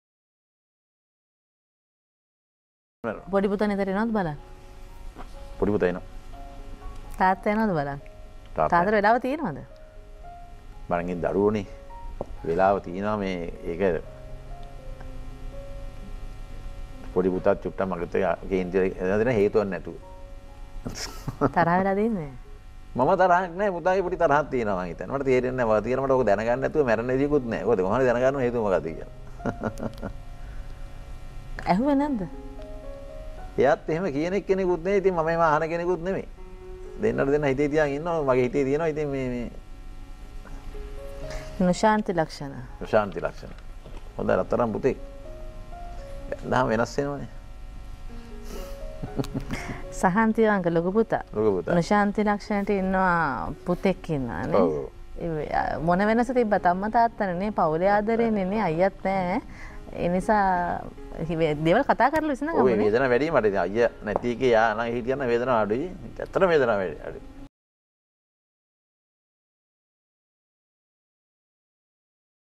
"මම තරහ නෑ.පුතාගෙ පොඩි තරහක් තියෙනවා හේතුවක් නැතුව. ඒක දැනගන්නෙ නැතුව මට මැරෙන්නත් බෑ" පාලිත තෙවරප්.
"මම තරහ නෑ.පුතාගෙ පොඩි තරහක් තියෙනවා හේතුවක් නැතුව. ඒක දැනගන්නෙ නැතුව මට මැරෙන්නත් බෑ" පාලිත තෙවරප්පෙරුම

කවදාවත් බිරිද හෝ සැමියා නිසා දෙමාපියන් අත් නොහරින්න. මන්ද ඔවුන් ජීවිතයම කැප කරන්නේ දරුවන් වෙනුවෙනි.

ඔවුන් ඔබෙන් වෙන්වූ පසු ඔබේ ජීවිත කාලයම ඒ වෙනුවෙන් දුක් වෙන්න වේවි.